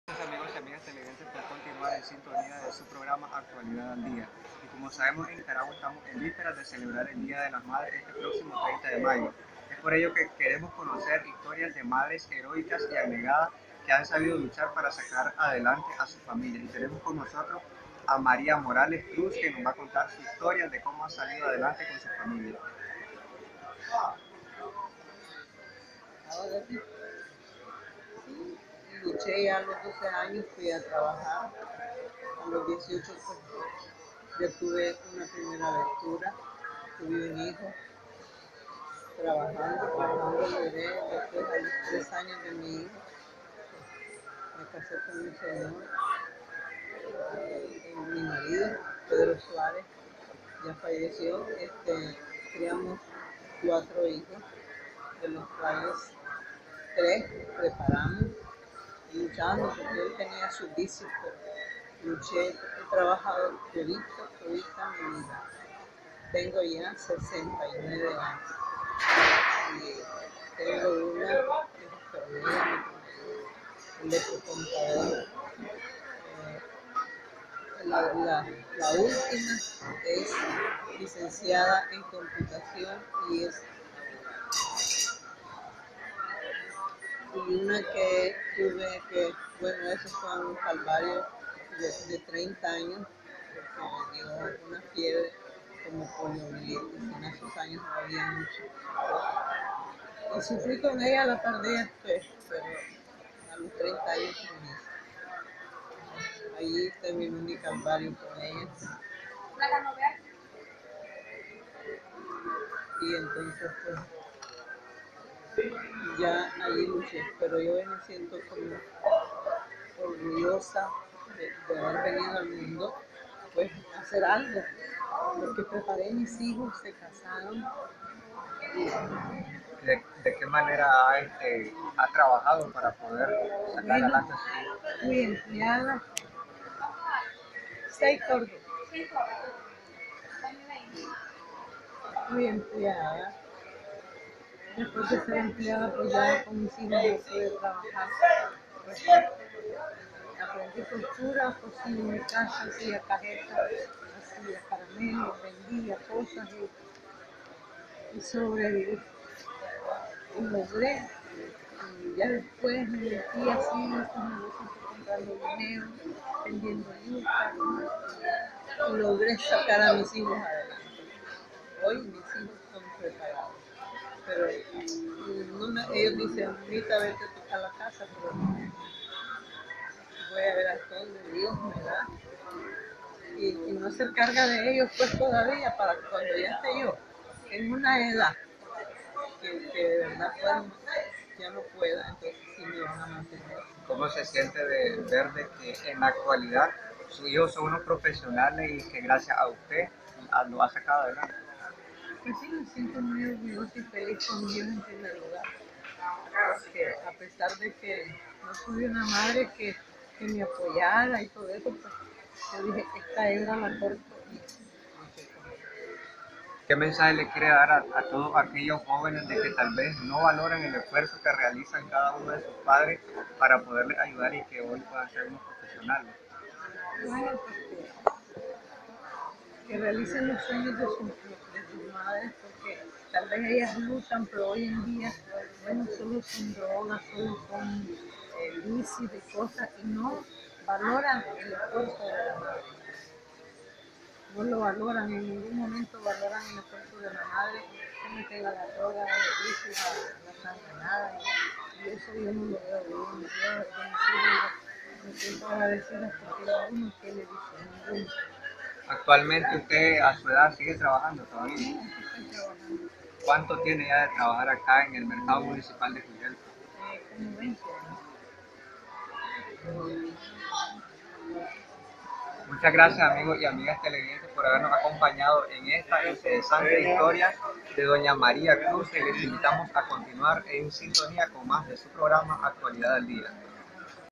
Entrevista a Maria Morales Cruz
Esta es una historia de esas mujeres heroicas que trabajan dia a dia para sacar adelante a sus familias.